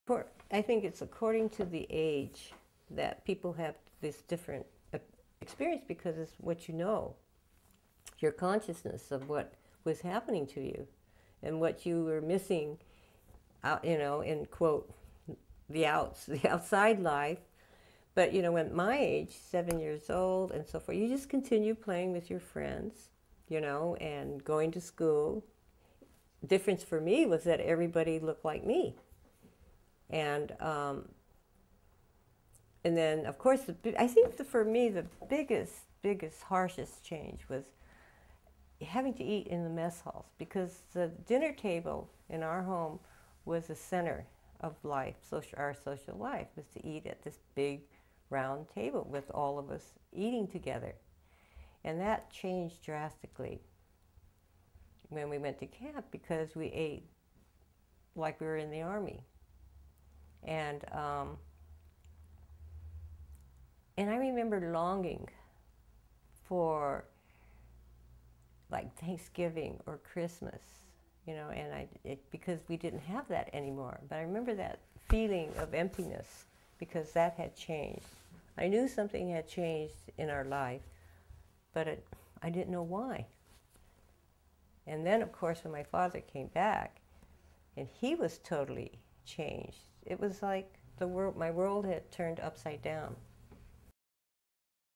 Jeanne Wakatsuki Houston #3: Initial impact on life at camp
Date: December 27, 2005
Location: California, US
Interviewer: John Esaki
Contributed by: Watase Media Arts Center, Japanese American National Museum